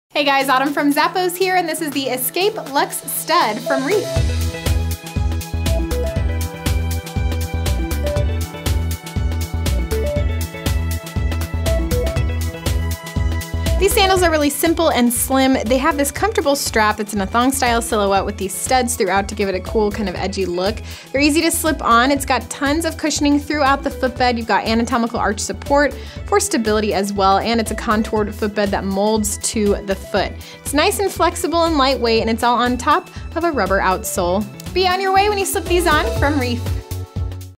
Reef Escape Lux Stud SKU: 9119324
Escape is a state of spirit, not a state of mind. Embrace the journey with the revolutionary comfort of the Reef&reg; Escape Lux Stud sandal!
Superior strap strength for long-lasting wear.
Metal stud embellishments along the strap.
Exclusive cushion rubber for all-day comfort.
Oval toe post offers ultimate comfort between the toes.
Enhanced anatomical arch support for stability and support.
Contoured footbed molds to your foot for a feminine silhouette.
100% PVC-free material.
Rubber outsole offers grippy performance and durability.
Imported.